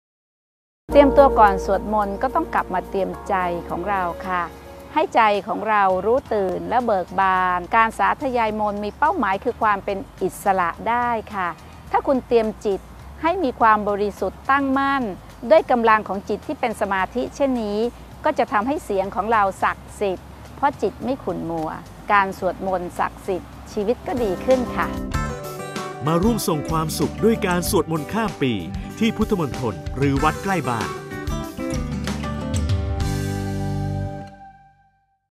สวดมนต์ข้ามปี แม่ชีศันสนีย์ เสถียรสุต_2
การสวดมนต์คือ การสร้างสิริมงคลให้ตัวเอง ส่งผลให้เราได้พบกับสิ่งที่ดี และเป็นสิริมงคลตลอดทั้งปี
มาร่วมส่งความสุข ด้วยสวดมนต์ข้ามปี ในคืนวันที่ 31 ธันวาคม ที่พุทธมณฑล และวัดใกล้บ้าน
วัดความสุขของคุณและดูข้อมูลเพิ่มเติมได้ที่

สำนักงานกองทุนสนับสนุนการสร้างเสริมสุขภาพ (สสส.)